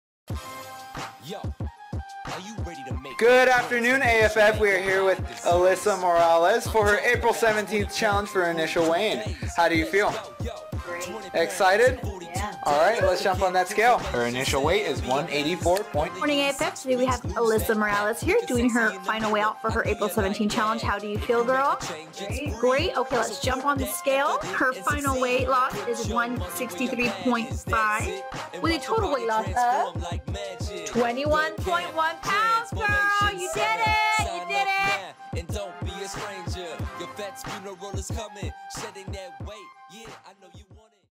Chino Hills Weight Loss Fitness 6 Week Challenge Results - Elisa Morales
We will now check the result of Elisa's 6 week weight loss challenge!